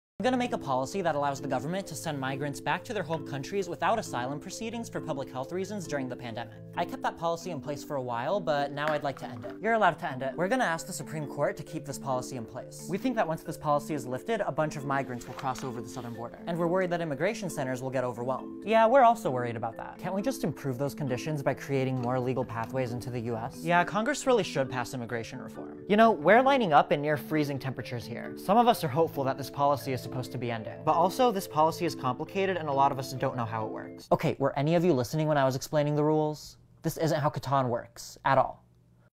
Supreme Court FREEZES order lifting border restrictions for migrants...
More than 2.4 million people have been expelled, mostly from the southern border, since the Trump administration imposed the order in March 2020, ostensibly to prevent the spread of the coronavirus, though advocates argued that he used the measure to slash immigration.